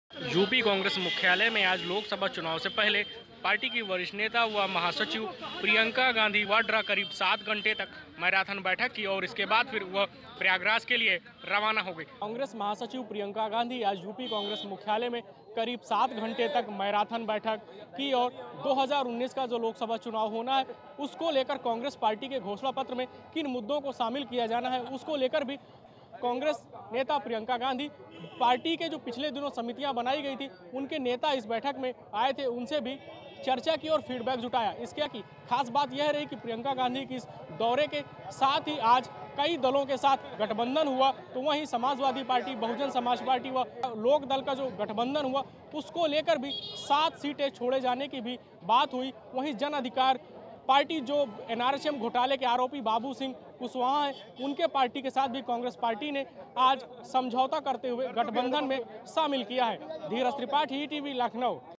सात घंटे की मैराथन बैठक के बाद प्रियंका गांधी प्रयागराज हुईं रवाना
लखनऊ: यूपी कांग्रेस मुख्यालय में आज राष्ट्रीय महासचिव प्रियंका गांधी करीब 7 घंटे तक लगातार मैराथन बैठक की. प्रियंका गांधी ने लोकसभा चुनाव की तैयारियों को लेकर पार्टी नेताओं के साथ चर्चा की और रणनीति बनाई.